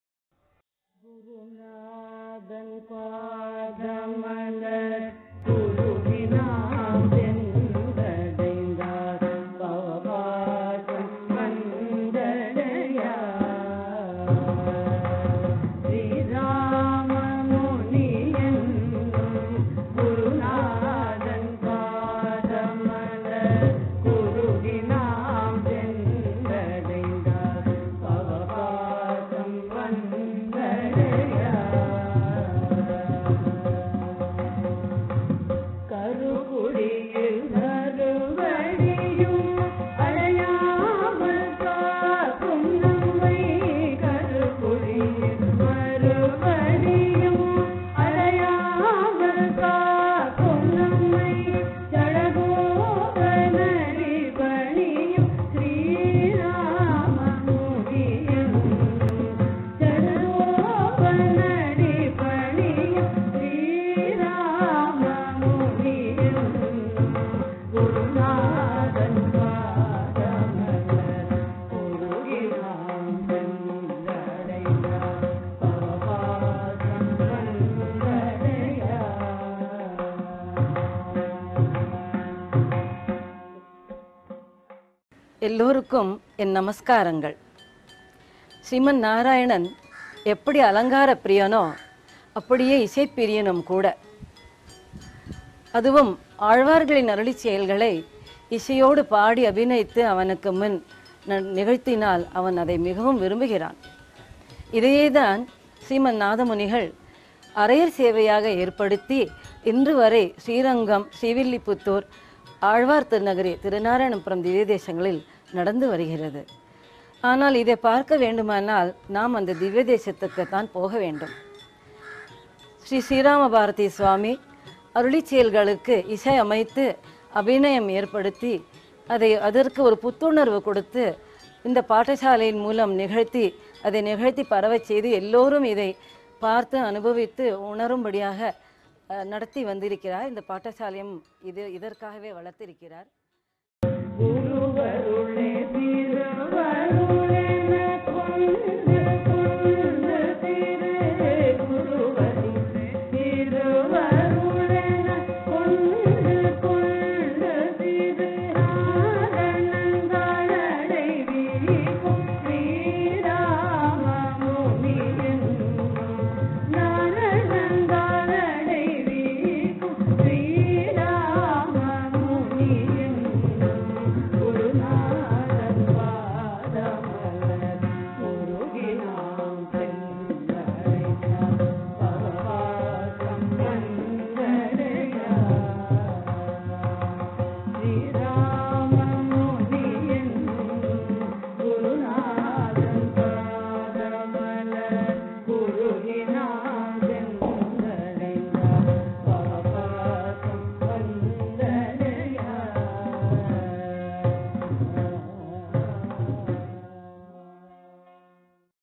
Aani-Ashwini-Bharani Utsavam - 2016
STDP Invites you to celebrate the tirunakshatram of Muddu Selvan and Swami Srirama Bharati .

Program Details
29-June-2016 -  Wednesday - Aani Ashwini
10 AM to 12 PM - Moolavar Tirumanjam
6 PM to 8 PM - Purappaadu and Perumal tirunakshatram celebrations

30-June-2016 - Thursday - Aani Bharani
10 AM to 12 PM - Tirumanjanam for utsavar and Sri Srirama Bharati
6 PM to 7 30 PM - Guru samarpanam by STD Pathasala students